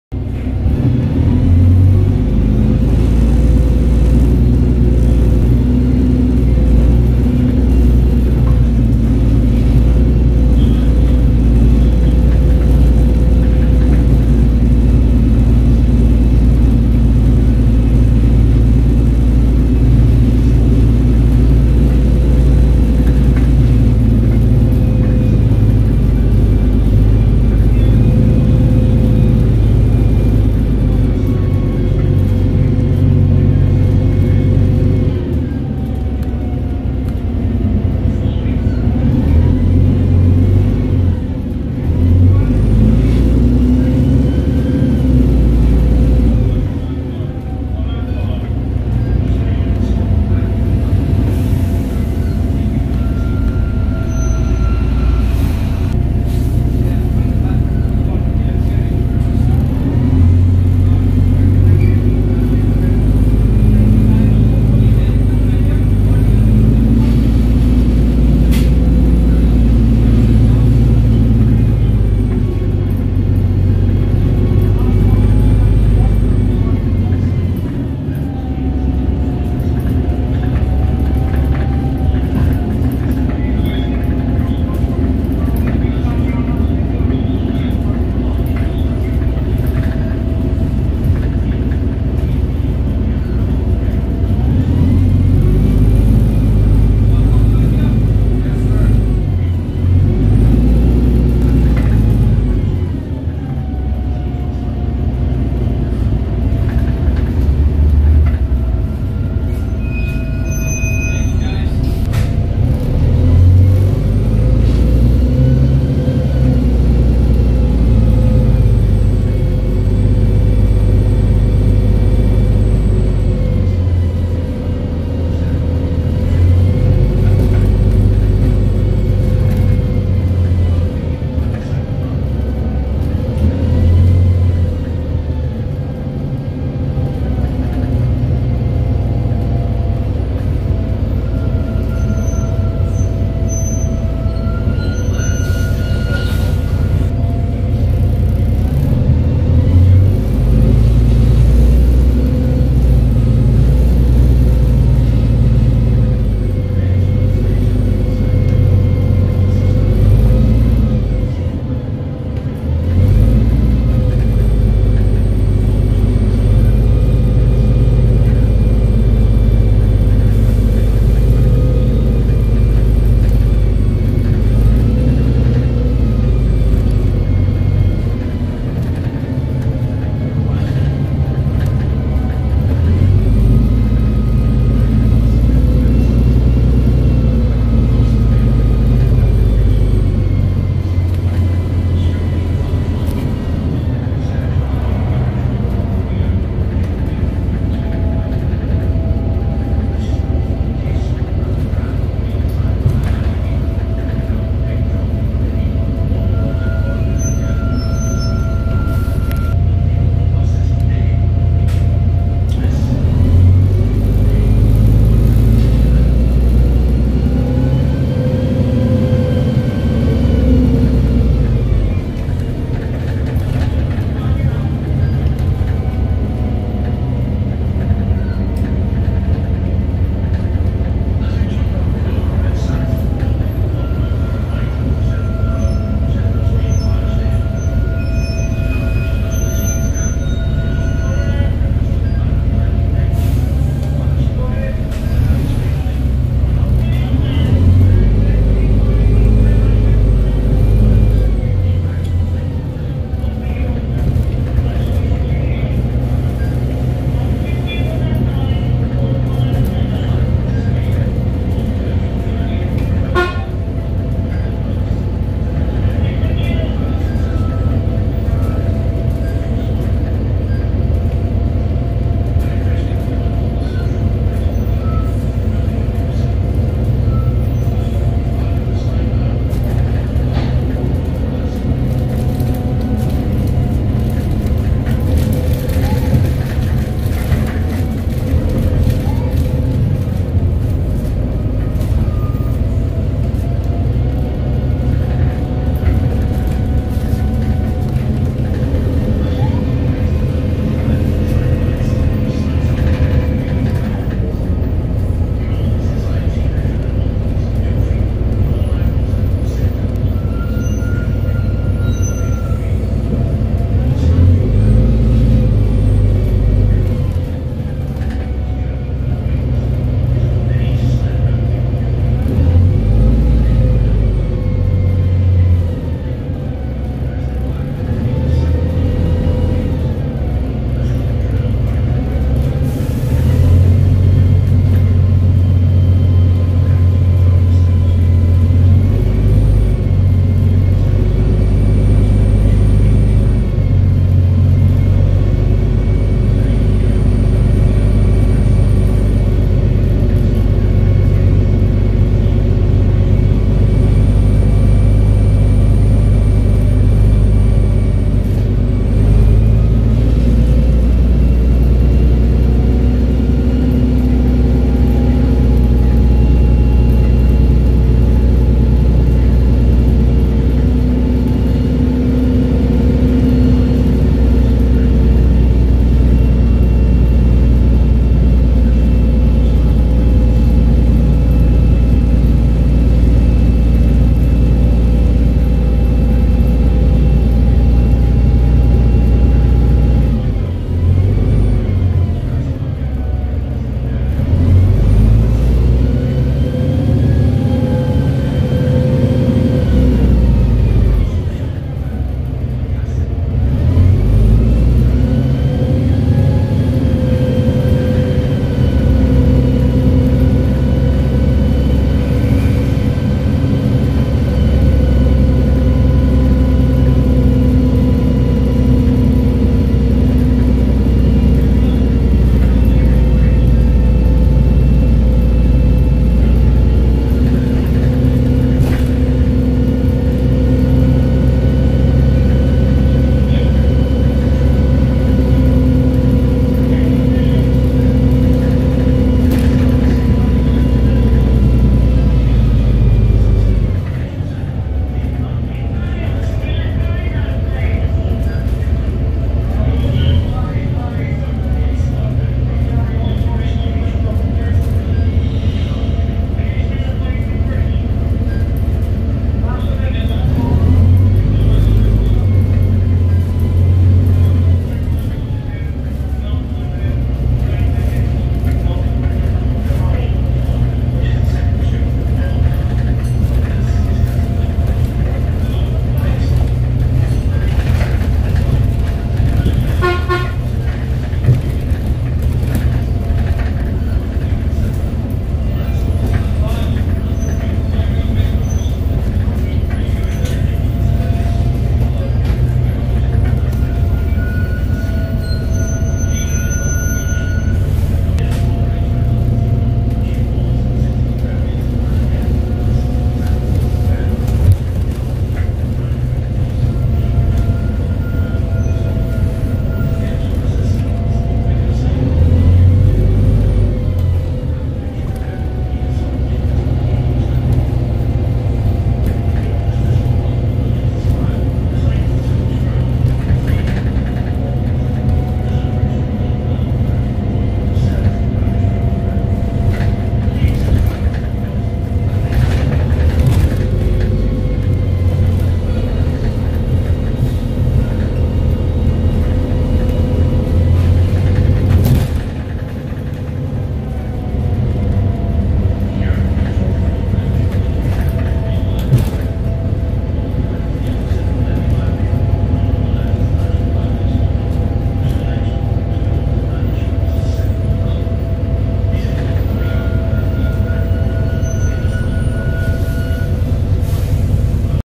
FSE/Bright Bus Tours DAF DB250 35013 T213 XVB
First Scotland East/Bright Bus Tours DAF DB250LF (Voith gearbox) now registered as MXZ 3386 part 1
04:58